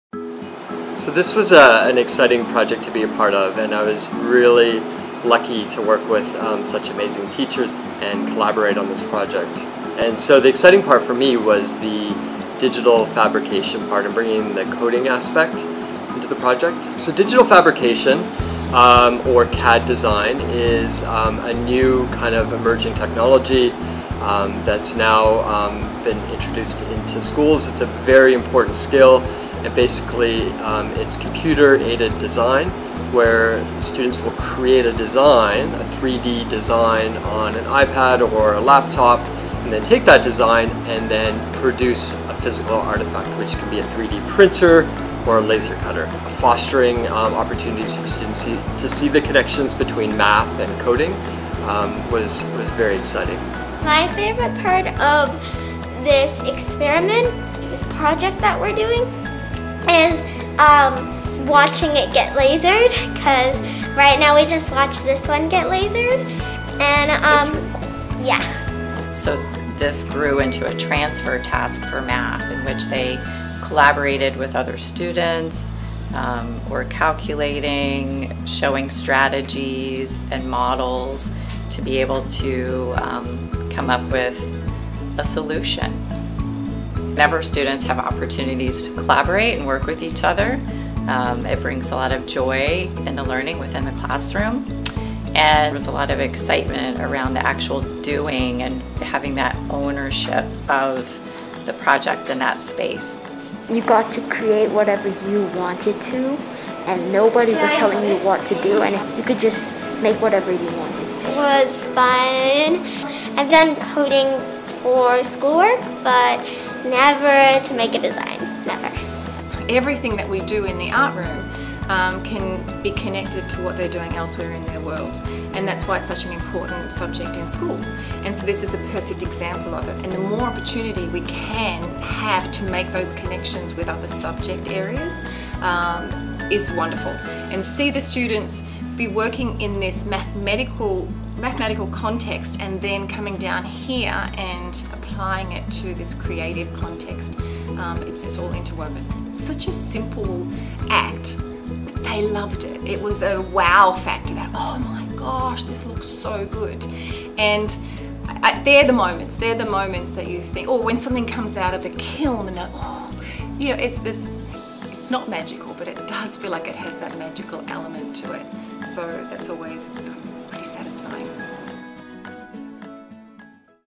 Create, Code and Connect with ISB Elementary School
This year our Grade 4 students learned to create, code, connect & collaborate through a design-thinking project that integrated Math, Art, and Coding. The goal was to help beautify the Grade 4 hallway and create a sense of community by displaying beautiful clay tiles that students designed, constructed, and painted.

Students were introduced to Thai radial designs in architecture and art while exploring our ISB Values and Attributes to find inspiration for their project. They then used coding to create their radial designs using an app called Turtle Art. Students were required to calculate the area and perimeter for the number of tiles and dimensions of the shapes to fit into the allocated wall space. Then the magic began; students' CAD designs were laser cut in the Invention Center using acrylic sheets to create shapes to imprint in their clay tiles, which they later personalized in the art room.

Through digital innovation and authentic design across multiple disciplines, this project is a true example of applied and transferrable learning at ISB.